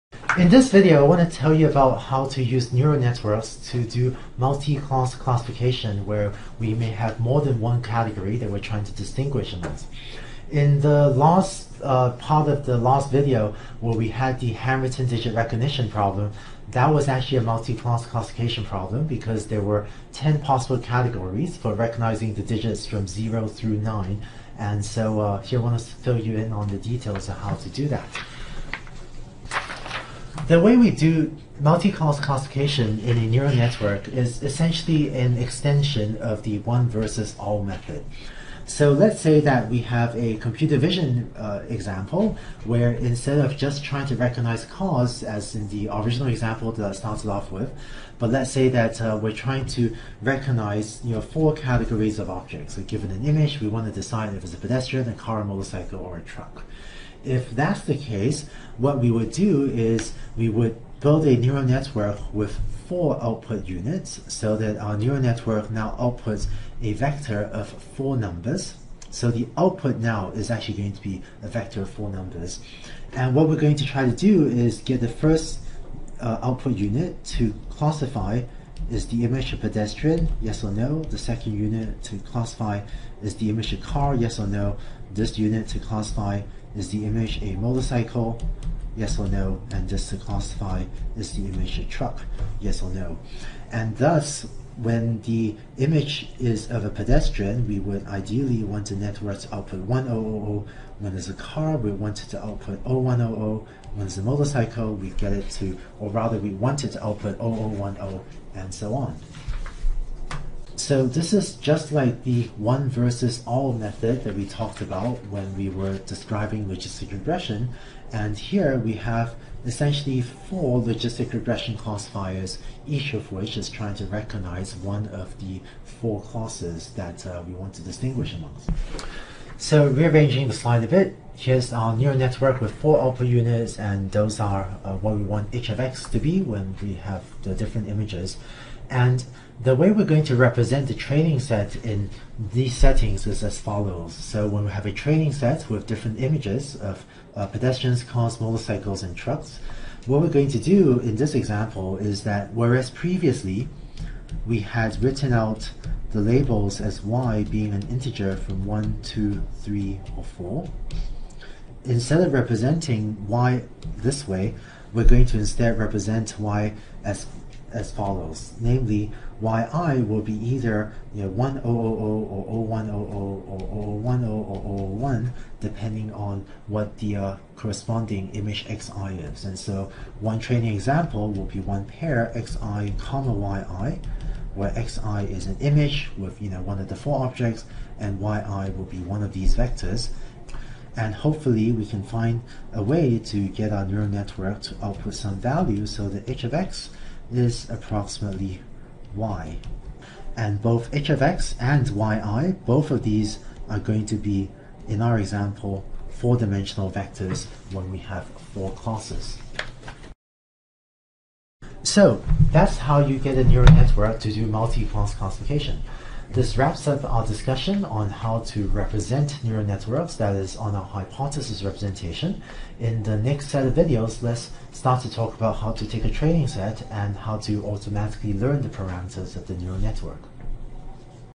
4.3.3 Neural Networks Multiclass Classification by Andrew Ng
Neural Networks : Representation

Machine Learning - Stanford University | Coursera
by Andrew Ng

Learn Machine Learning for free.